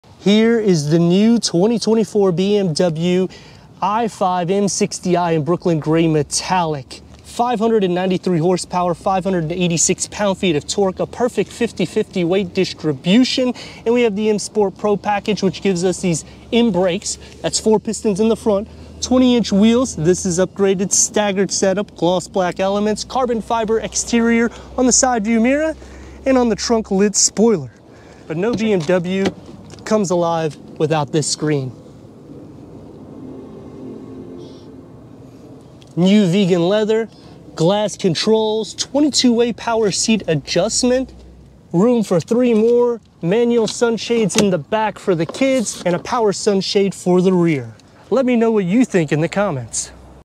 2024 BMW i5 m60 Quick Look /
Here is the 2024 BMW i5 m60 in this shorts review I will go over the inside and outside. Join me for full reviews @HawkeyeRides